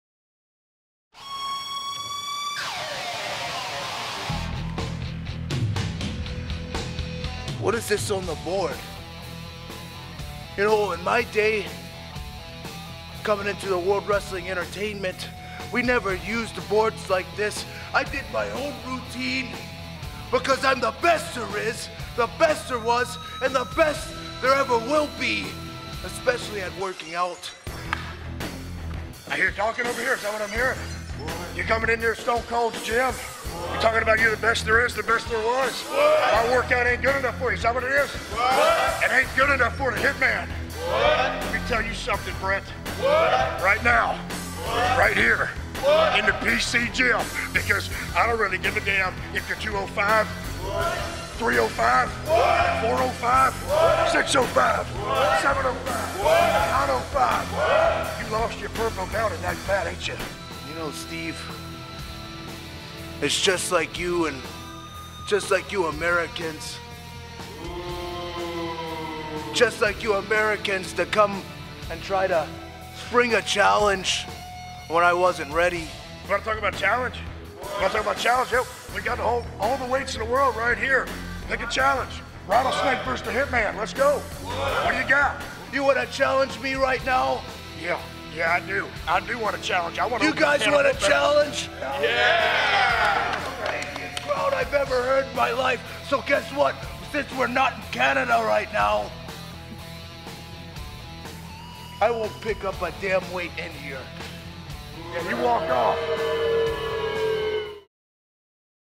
Rich Swann and Sean Hayes reenact Bret Hart-"Stone Cold" confrontation inside WWE Performance Center
One of WWE's most historic rivalries, Bret "Hit Man" Hart vs. "Stone Cold" Steve Austin, comes alive again in the WWE Performance Center, courtesy of Rich Swann and Strength & Conditioning Coach Sean Hayes.